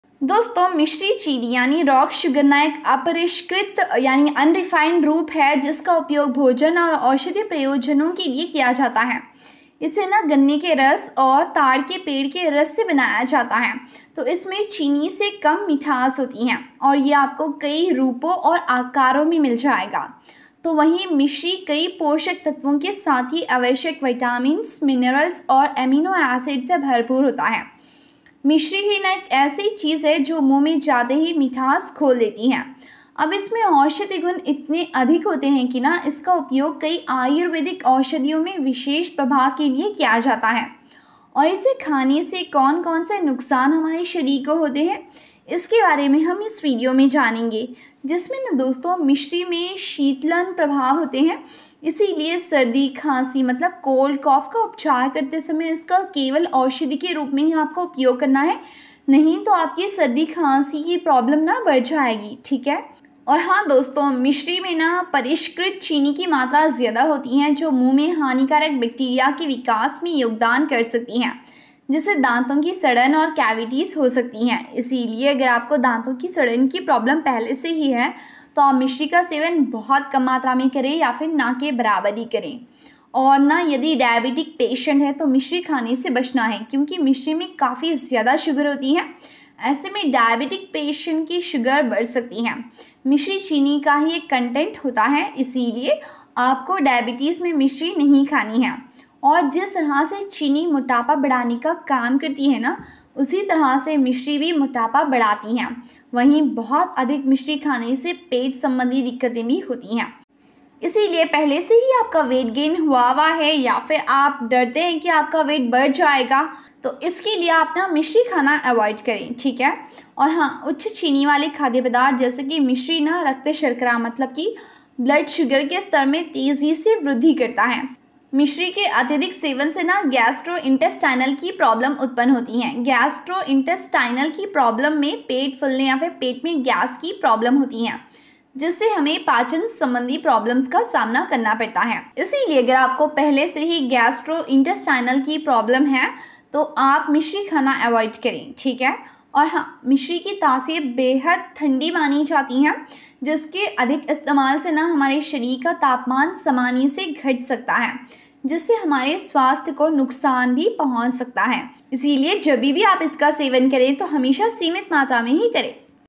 रोजाना मिश्री खाने से क्या होता है | मिश्री खाने के नुकसान जानकर रह जाएंगे दंग
रोजाना मिश्री खाने से क्या होता है | मिश्री खाने के नुकसान जानकर रह जाएंगे दंग|| Disadvantanges of eating sugar